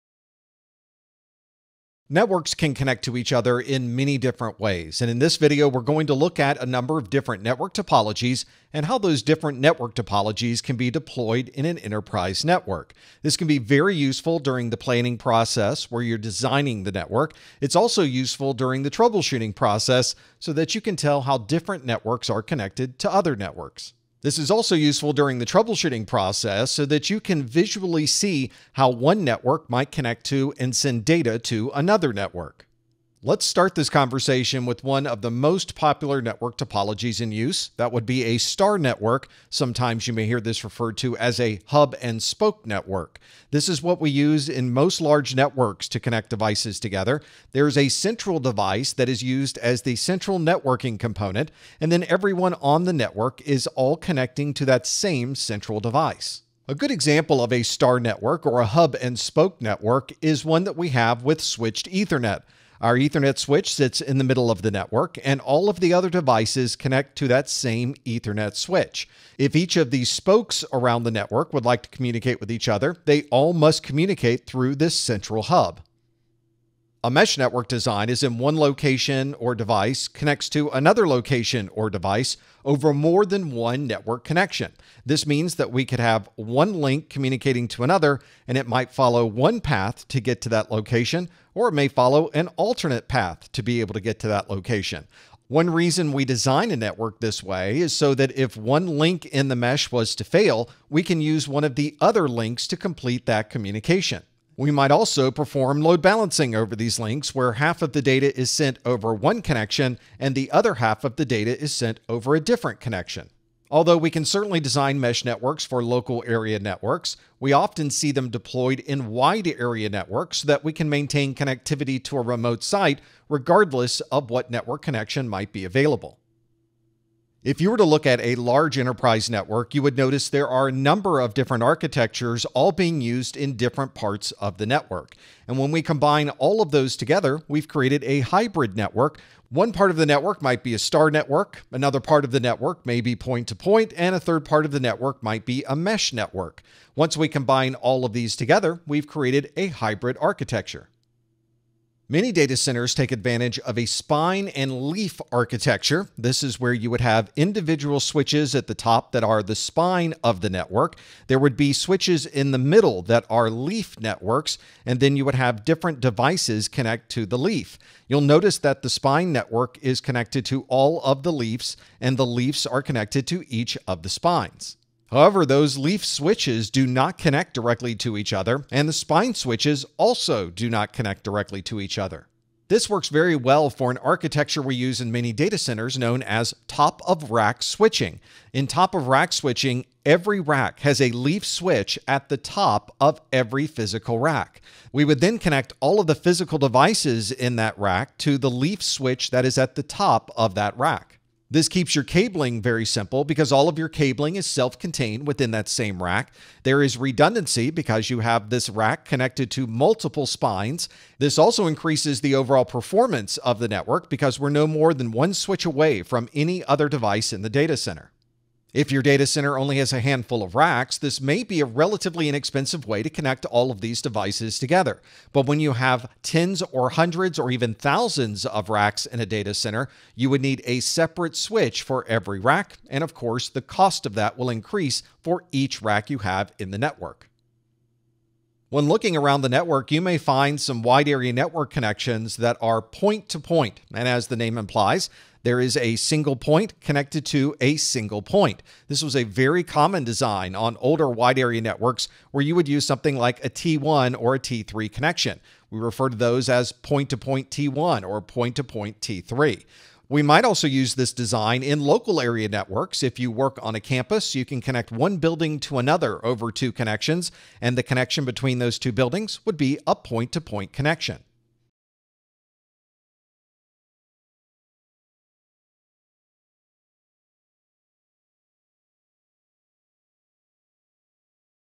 Network Topologies - CompTIA Network+ N10-009 - 1.6
We often use broad descriptions when describing network topologies. In this video, you'll learn about star, mesh, hybrid, spine and leaf, and other network topology types.